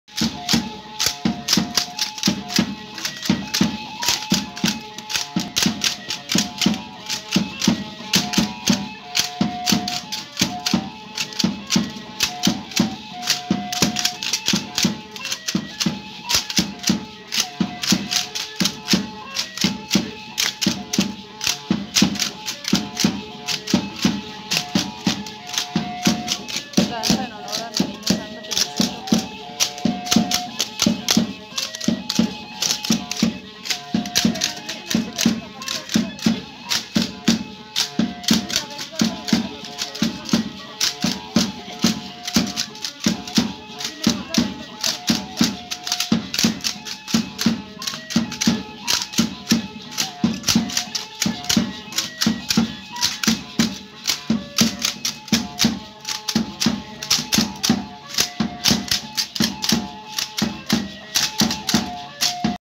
danza al niño fidencio
asi las fiestas en espinazo en honor al niño fidencio , si no crees no ensucie mi publicacion